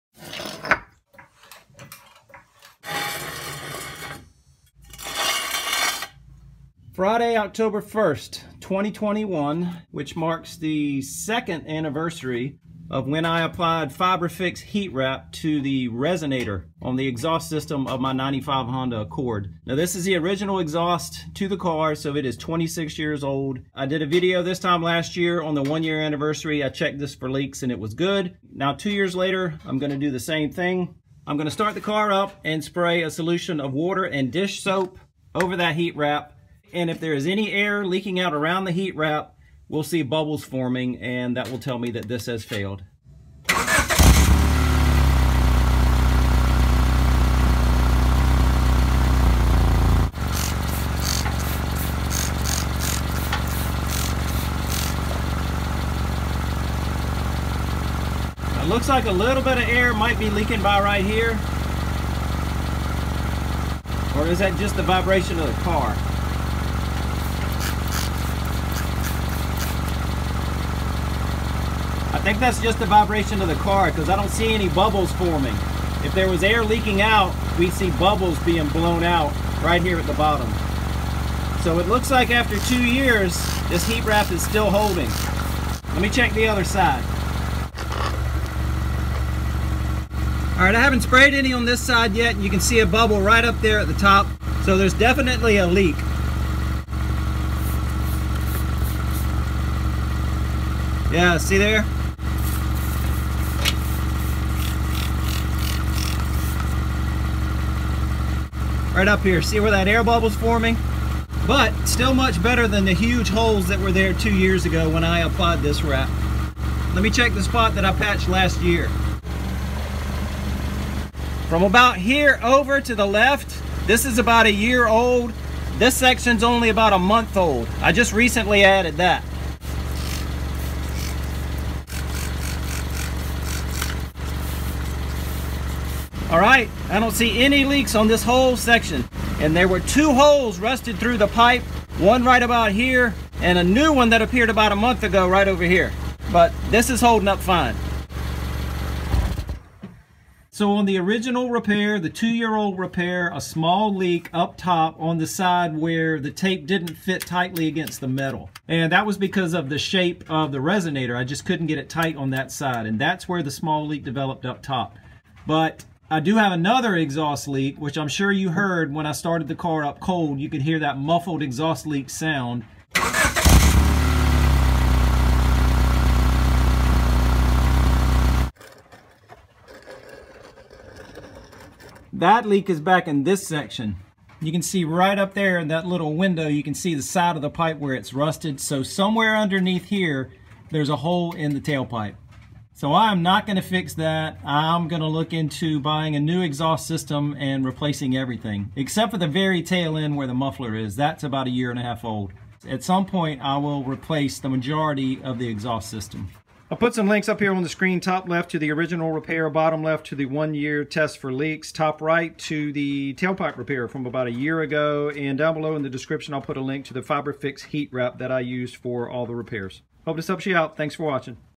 FiberFix Exhaust Repair 2 Years Later
Two year check of FiberFix Heat Wrap exhaust repair on my 1995 Honda Accord..